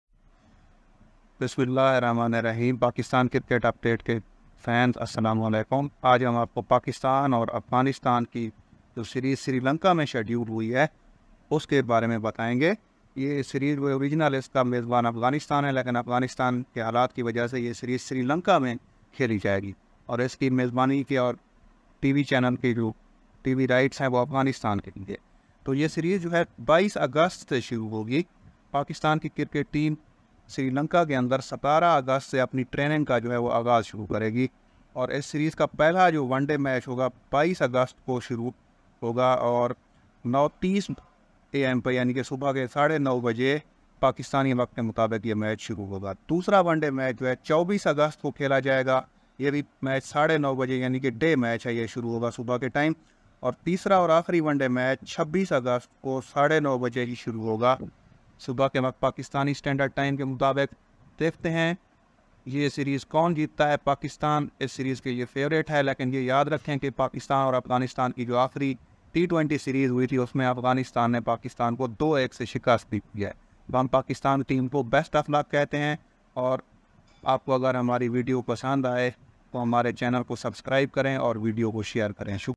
Cricket Clash: Pakistan vs. Afghanistan ODI Series 2023 | Full Schedule Revealed!
Get ready to witness an exciting cricket series played at the picturesque venues of Sri Lanka. The Pakistan vs. Afghanistan ODI Series 2023 is all set to enthrall cricket fans from 22nd August to 26th August, featuring three intense ODI matches.

Sri Lanka, with its stunning grounds and passionate cricket-loving crowd, will be the host for this thrilling series. The schedule is as follows:

1st ODI: 22nd August 2023 - The series kicks off with a high-octane clash between Pakistan and Afghanistan at a Sri Lankan venue. The players will bring their A-game to set the tone for the rest of the series.

2nd ODI: 24th August 2023 - The action continues as the teams battle it out in another exciting encounter on Sri Lankan soil. Expect nail-biting moments and extraordinary performances from both sides.

3rd ODI: 26th August 2023 - The final match of the series will take place at yet another iconic Sri Lankan ground. Emotions will run high as the teams strive to claim the series victory.

We will be providing comprehensive coverage of the series, including match highlights, player interviews, and behind-the-scenes glimpses. Don't miss any of the thrilling moments from this epic cricket showdown.

So, mark your calendars and join us in supporting your favorite team as they vie for supremacy in this enthralling ODI series played in the breathtaking settings of Sri Lanka. Let's celebrate cricket together!